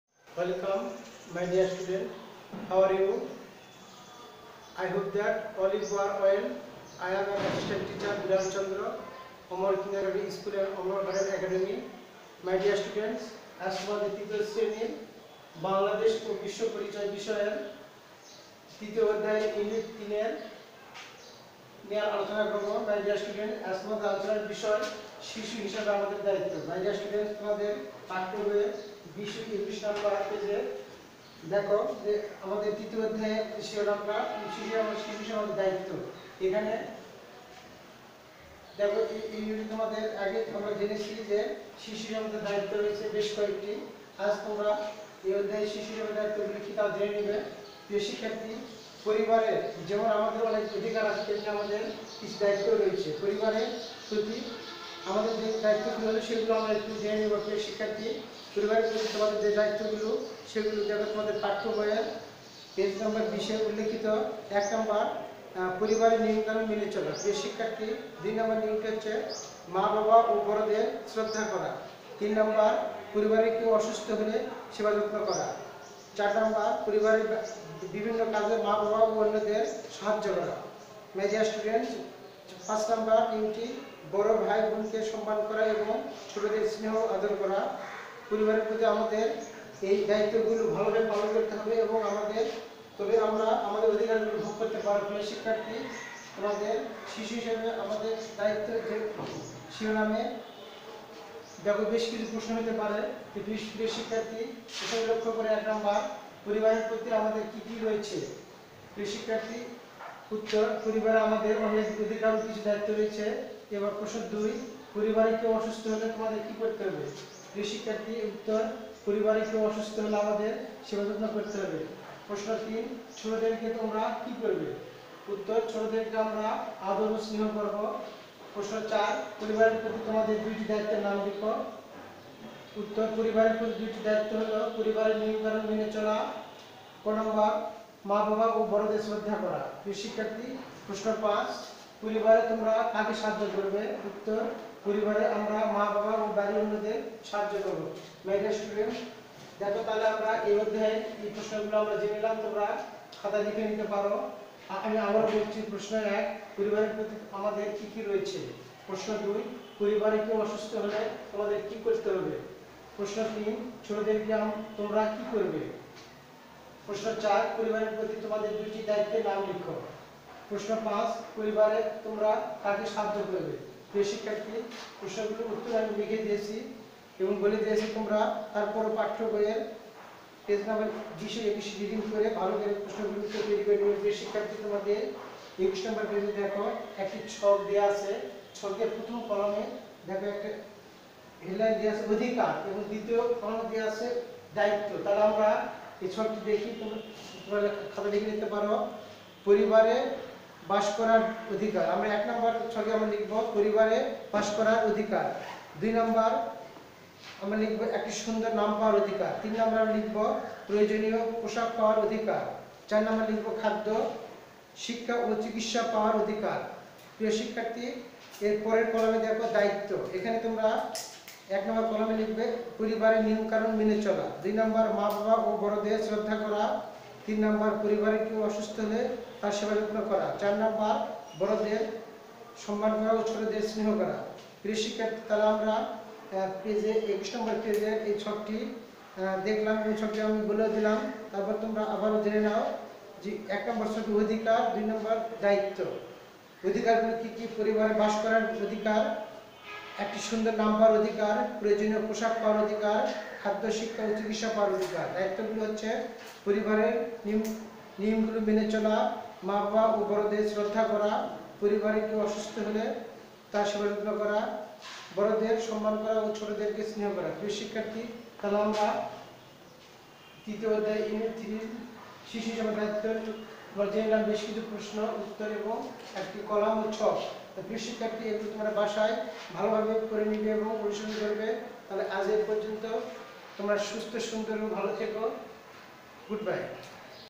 তৃতীয় শ্রেণি, বাংলাদেশ ও বিশ্বপরিচয় অনলাইন ক্লাস
শ্রেণিভিত্তিক অনলাইন ক্লাস: (তৃতীয় শ্রেণি) বাংলাদেশ ও বিশ্বপরিচয়, অধ্যায়: তিন (শিশু হিসাবে আমাদের দ্বায়িত্ব)